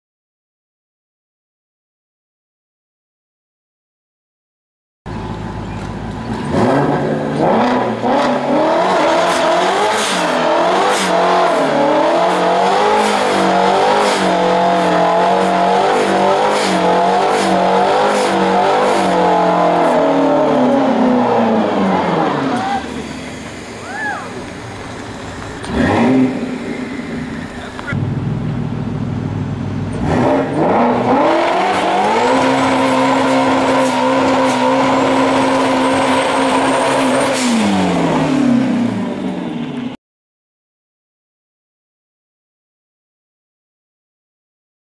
Ford Shelby GT350 Burnout Twin Turbo
Tony's 2016 Shelby - Burnouts for Murrica!
Custom Twin Turbo by FatFab

Tony's Mustang Turbo setup is all 100% hand built here in house.  We provide everything from parts sales, custom fabrication, installation and dyno tuning.

Basic Mods

Fathouse Fab Mustang Turbo Kit
Precision 6266 Dual Ball Bearing Turbos
Borla ATAK Active Catback
ID 1000 Injectors
Tuned on Flex Fuel
836hp on E85 and 8psi!
Custom Dyno Tune by Johnson Tuning
Tial Sport Wastegate and BOV
Custom Trans Cooler and Diff Cooler
Shelby R Brakes
Forgeline Wheels
McLeod RXT Clutch
Full BMR Suspension

This GT350 twin turbo kit makes 833 horsepower on only 8 psi of boost!